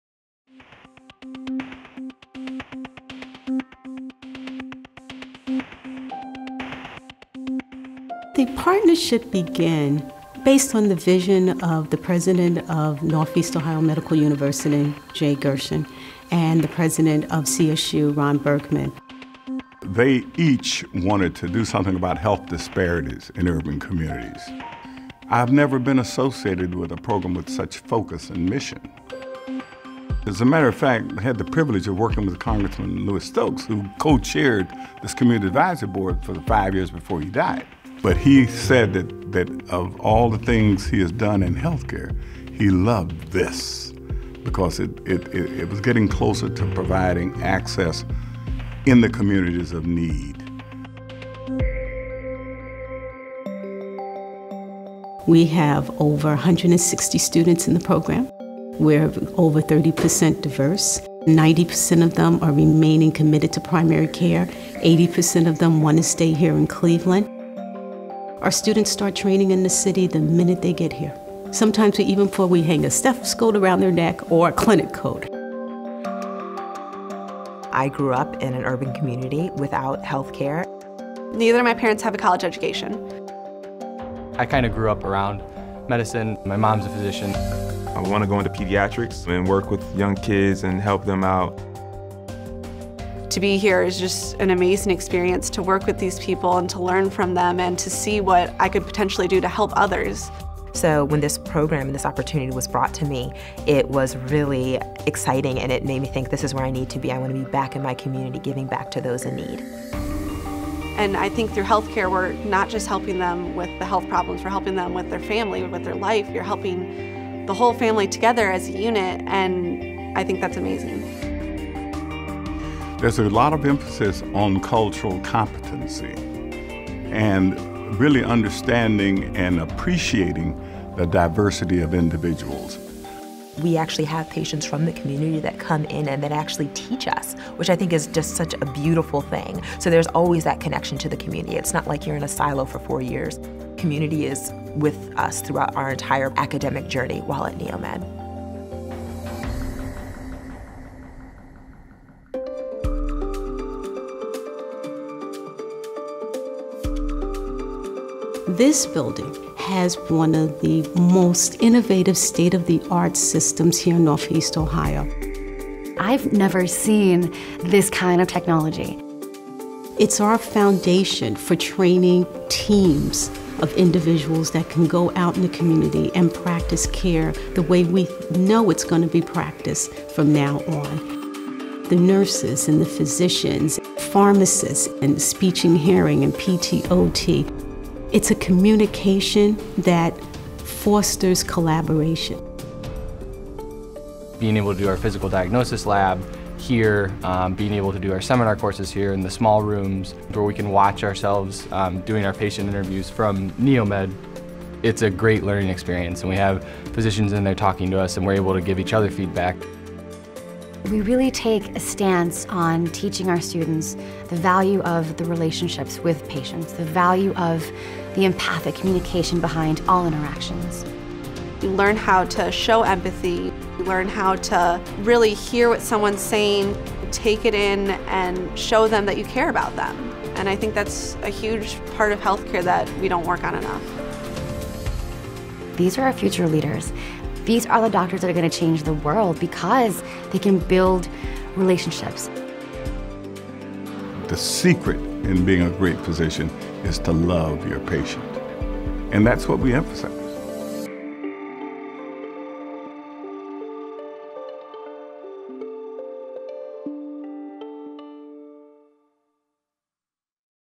The Center for Innovation in Medical Professions
Cleveland State University's Center for Innovation in Medical Professions (CIMP) houses the NEOMED-CSU Partnership for Urban Health and is a hub for inter-professional training and practice.

To learn more about the partnership between CSU and Northeast Ohio Medical University,